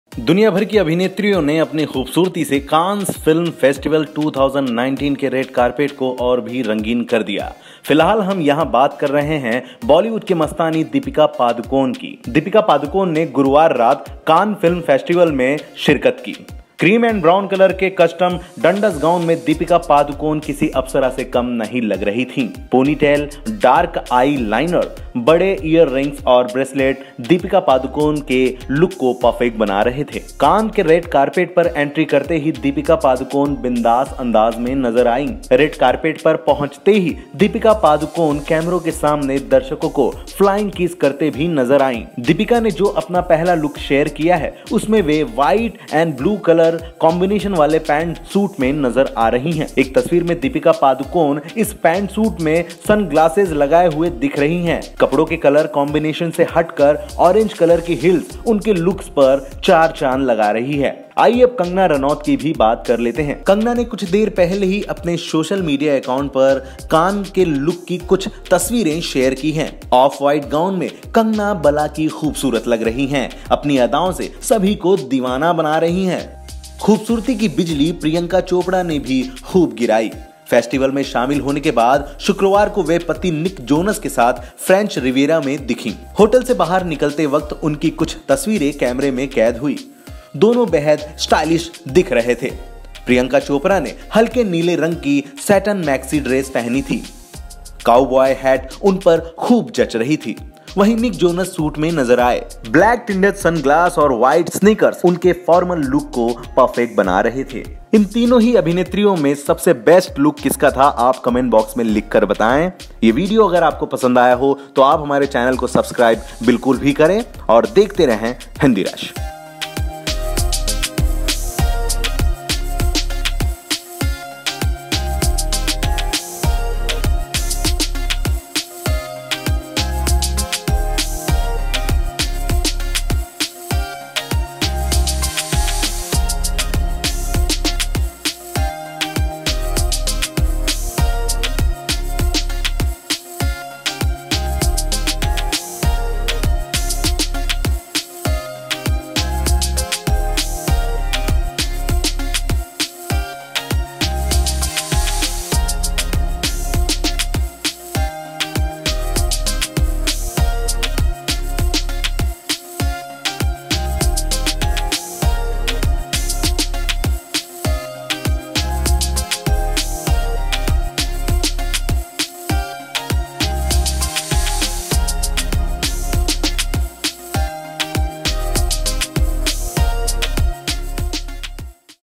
Cannes 2019: Deepika Padukone का लुक Best है या Priyanka Chopra, Kangana Ranaut का | Cannes Day 2
Cannes Film Festival 2019 में Deepika, Priyanka और Kangana Ranaut ने भी शिरकत की। ये तीनो ही अभिनेत्रियां बला की खूबसूरत लग रही थीं। Video देखें और बताएं आपको किसका लुक सबसे बेहतरीन लगा। इस बार ये फेस्टिवल 25 मई तक चलेगा। 72वें ‘कान्स फिल्म फेस्टिवल’ (Cannes 2019) में दुनियाभर की 21 दमदार फिल्मों के बीच मुकाबला होगा।